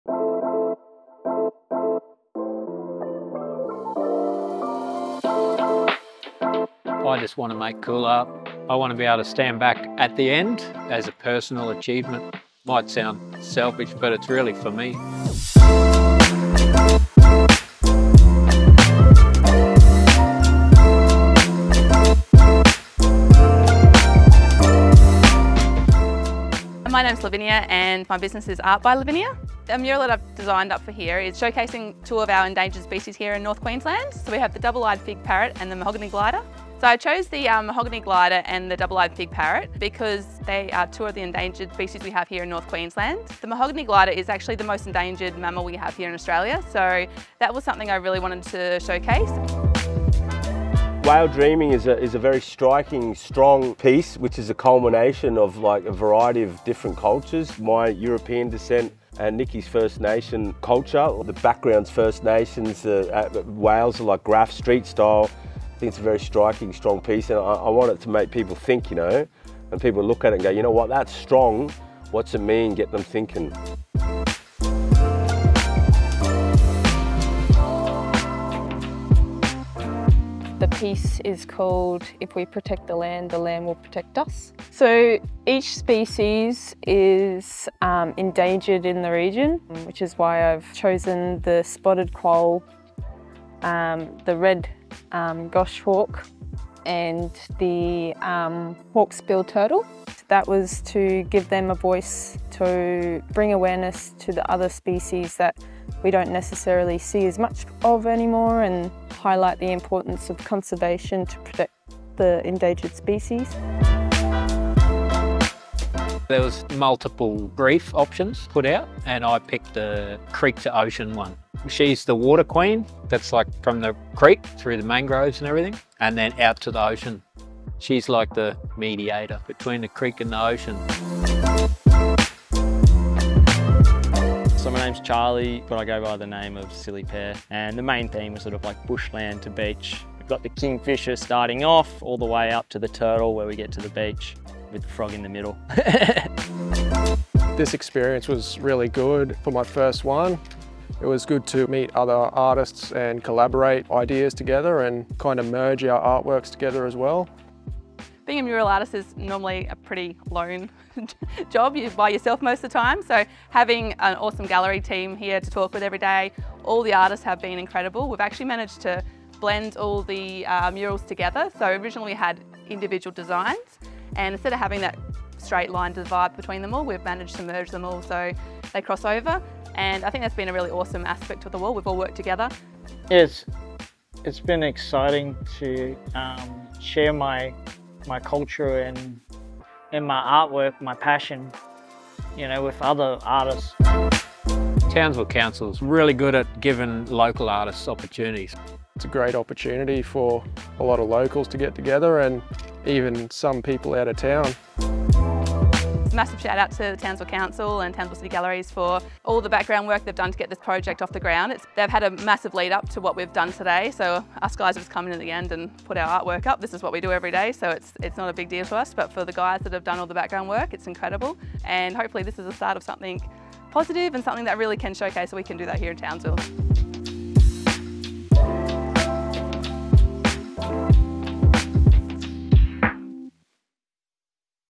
Street Art Walkway - Thuringowa Central
Seven artists have teamed up to create Townsville’s biggest piece of street art at a popular walkway located off High Range Drive, Thuringowa Central.

Local artists Alicia Allen, Art by Lavinia, Baret one., Mitchell Overest, Nicky Bidju Pryor and The Smizler were joined by NSW artist Silly Pear for the mammoth undertaking, covering two 43m and 83m-long walls.

The Townsville City Council funded project has provided provide a much-needed facelift for the walkway which connects High Range Drive and Gregory Street at Condon.